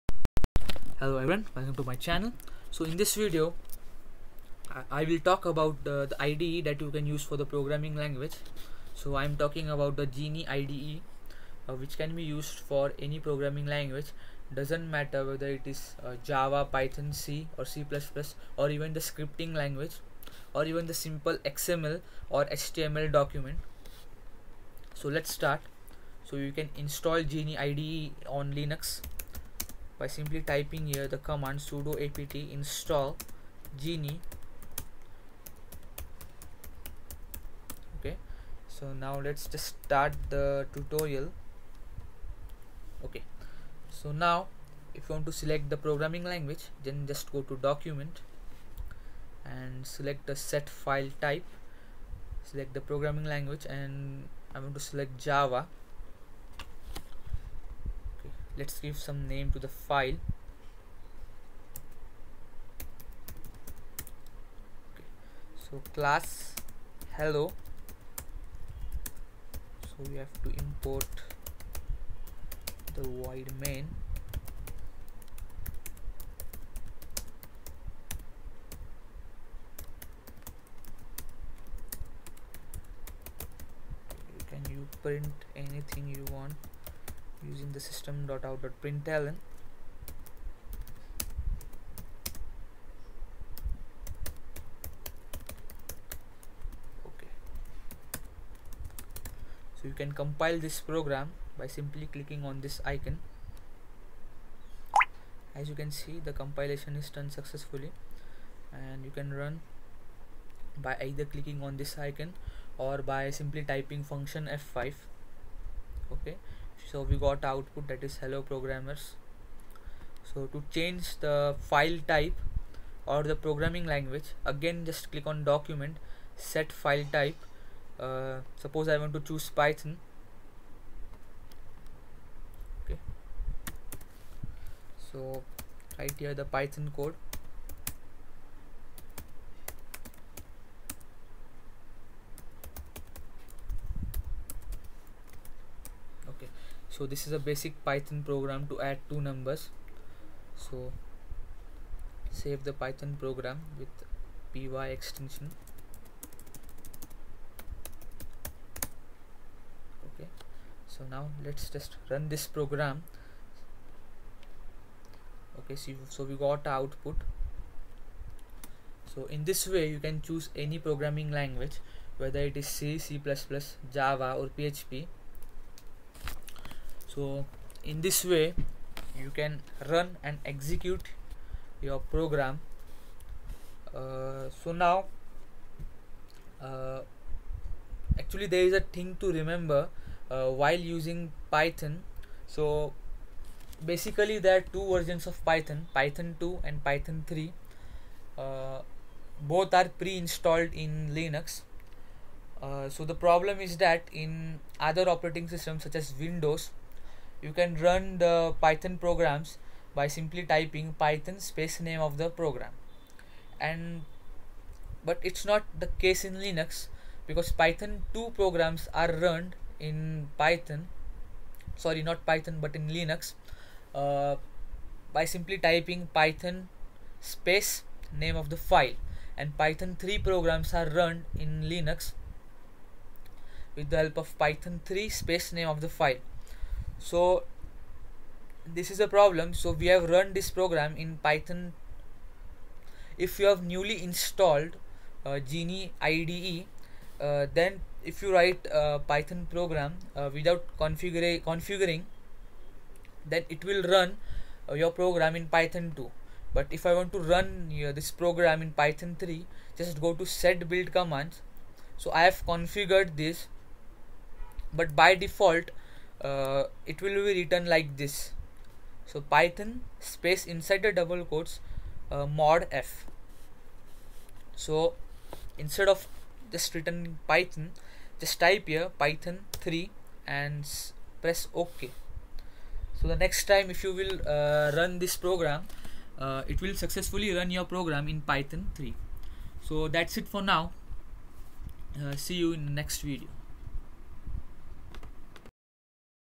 Geany IDE for linux
This video is about How to use Geany IDE  for different programming languages. Geany is IDE which supports programming languages,scripting languages and markup languages such as html and xml